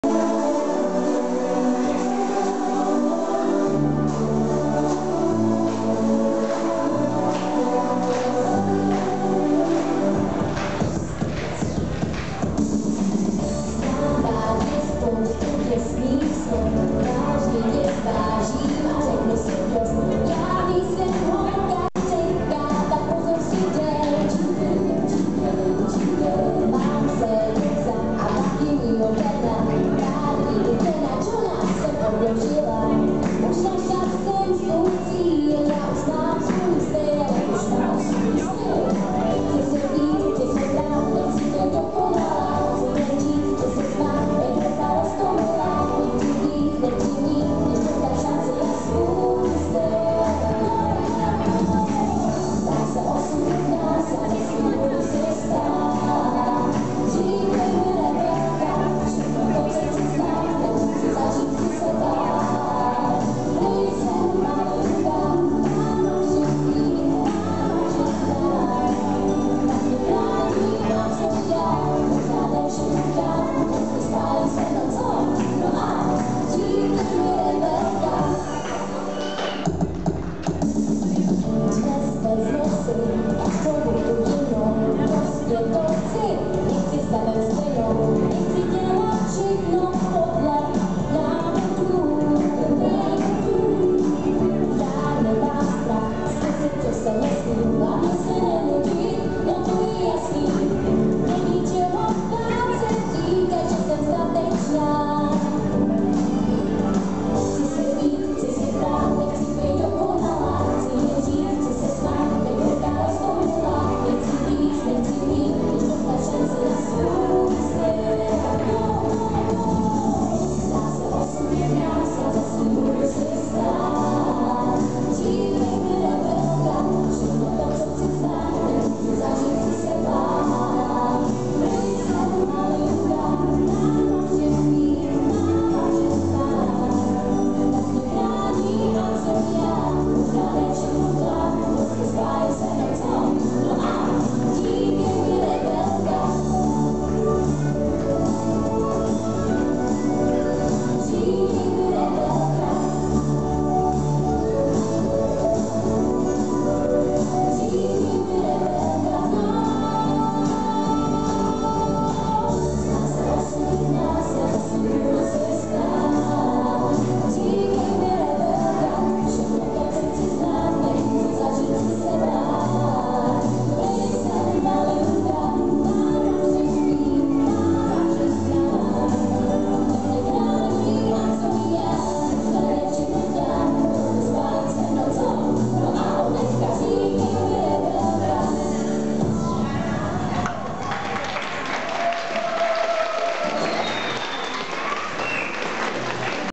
REBELKY - Dětský karneval 2016
Taneční skupina Rebelky z Březníka na Dětském karnevale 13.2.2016